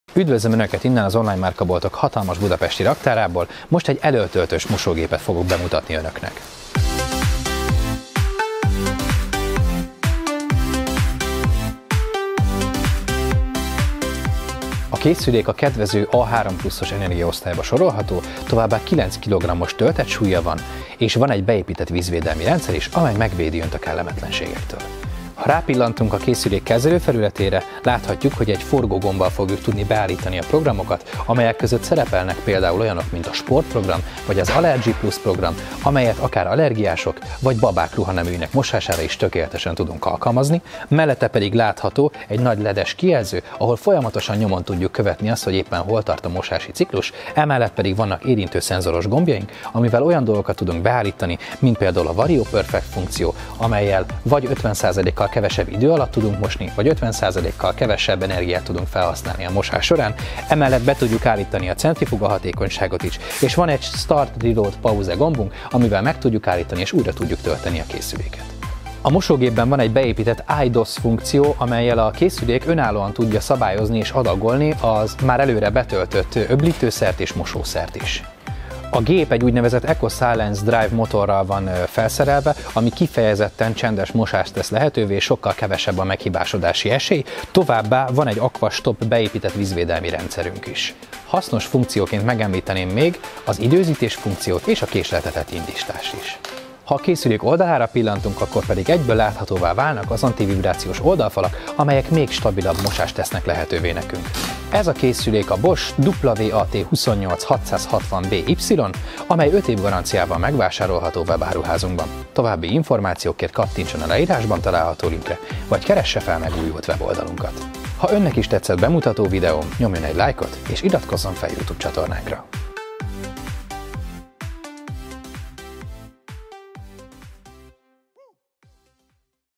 Bosch WAT28660BY | Elöltöltős mosógép bemutató videó
A Bosch WAT28660BY Elöltöltős mosógép 5 év garanciával megvásárolható a BOSCH Online Márkaboltban. További információkért kattintson a következő linkre:

A Bosch WAT28660BY Elöltöltős mosógép leghasznosabb tulajdonságai
►VarioPerfect™ opció
►SpeedPerfect, EcoPerfect
►ActiveWater™ technológia
►EcoSilence Drive™ motor